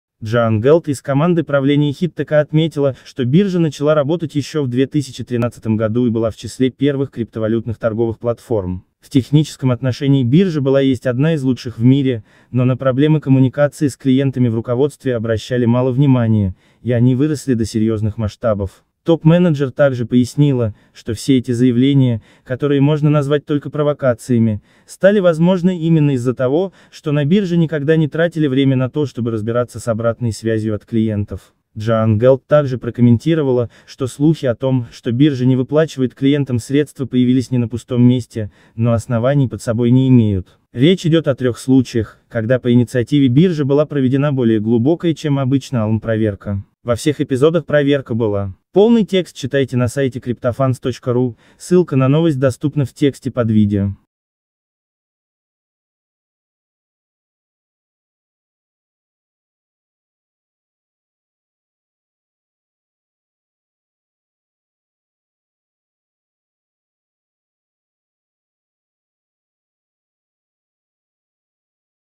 HitBTC отреклась от слухов о проблемах
Криптобиржа HitBTC категорически отрицает все проблемы, которые ей приписывают в связи с ее неплатежеспособностью.  Подобные обвин